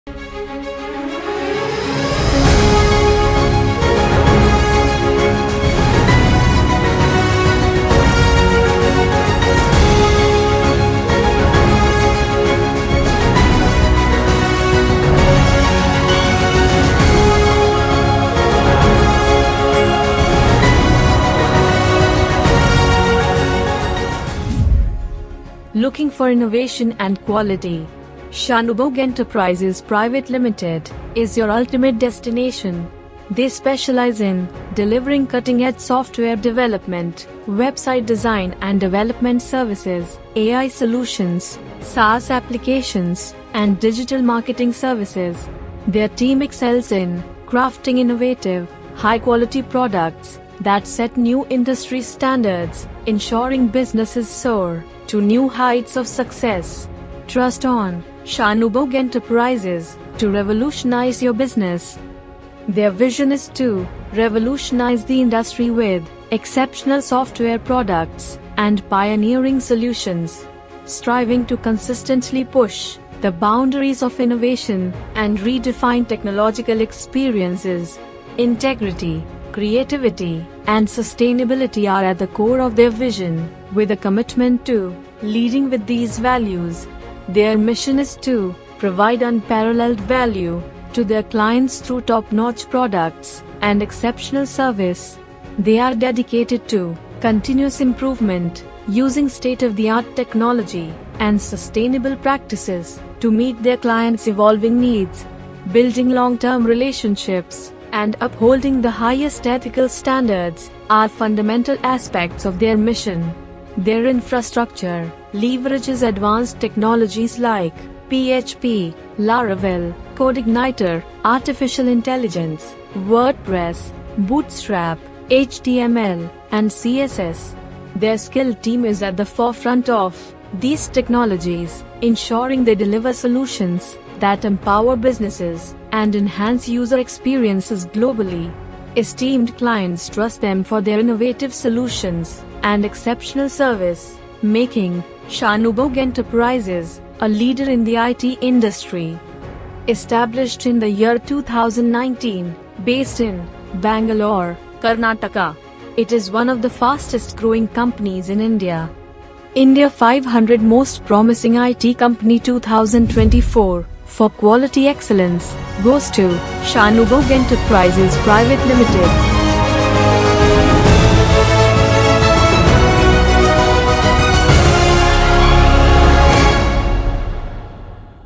Shanubogh Enterprises Pvt. Ltd. - India 500 Most Promising IT Company 2024.
Looking for innovation and quality? Shanubogh Enterprises Pvt. Ltd. is your ultimate destination. They specialize in delivering cutting-edge software development, website design and development services, AI solutions, SaaS applications, and digital marketing services. Their team excels in crafting innovative, high-quality products that set new industry standards, ensuring businesses soar to new heights of success. Trust Shanubogh Enterprises to revolutionize your business!

Their vision is to revolutionize the industry with exceptional software products and pioneering solutions, striving to consistently push the boundaries of innovation and redefine technological experiences. Integrity, creativity, and sustainability are at the core of their vision, with a commitment to leading with these values. Their mission is to provide unparalleled value to their clients through top-notch products and exceptional service. They are dedicated to continuous improvement, using state-of-the-art technology and sustainable practices to meet their clients' evolving needs. Building long-term relationships and upholding the highest ethical standards are fundamental aspects of their mission.

Their infrastructure leverages advanced technologies like PHP, Laravel, CodeIgniter, Artificial Intelligence, WordPress, Bootstrap, HTML, and CSS. Their skilled team is at the forefront of these technologies, ensuring they deliver solutions that empower businesses and enhance user experiences globally. Esteemed clients trust them for their innovative solutions and exceptional service, making Shanubogh Enterprises a leader in the IT industry.

Established in the year 2019, based in, Bangalore, Karnataka. It is one of the fastest-growing companies in India.

India 500 Most Promising IT Company 2024, For Quality Excellence, Goes to, Shanubogh Enterprises Pvt. Ltd.